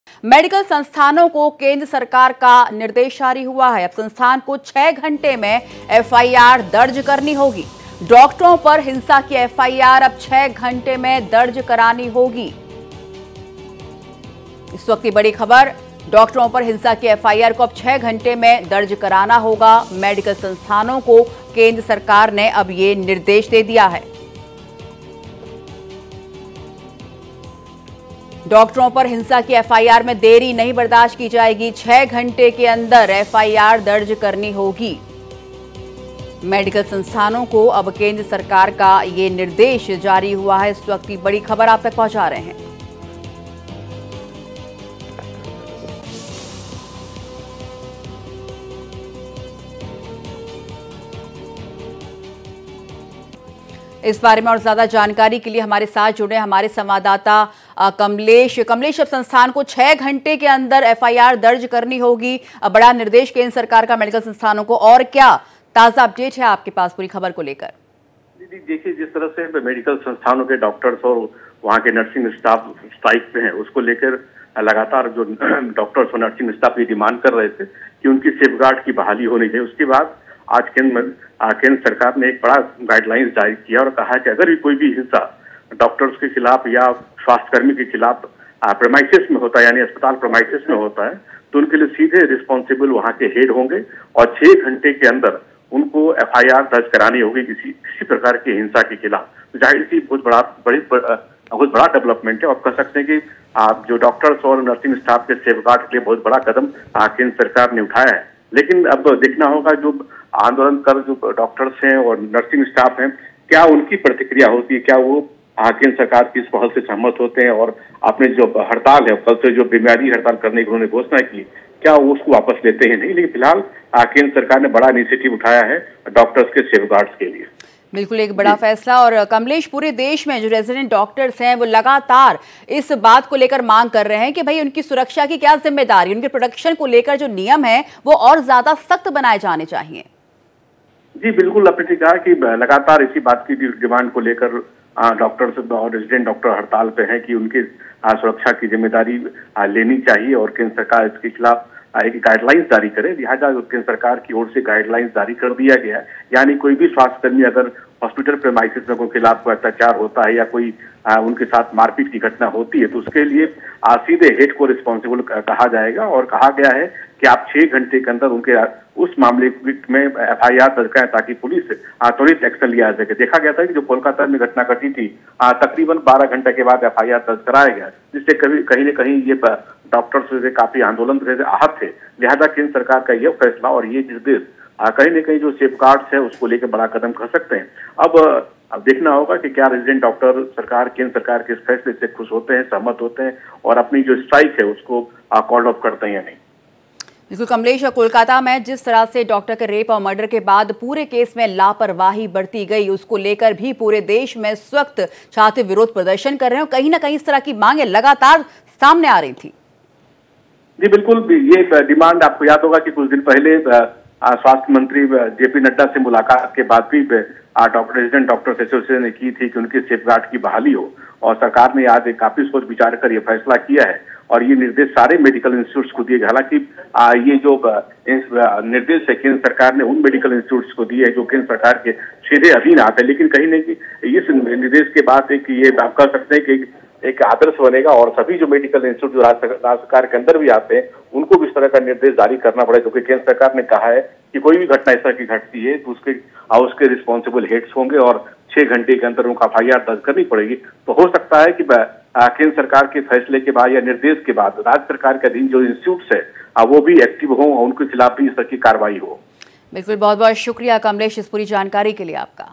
Breaking News : मेडिकल संस्थानों को केंद्र सरकार का निर्देश, अब 6 घंटे में दर्ज करानी होगी FIR
Ronak Rathi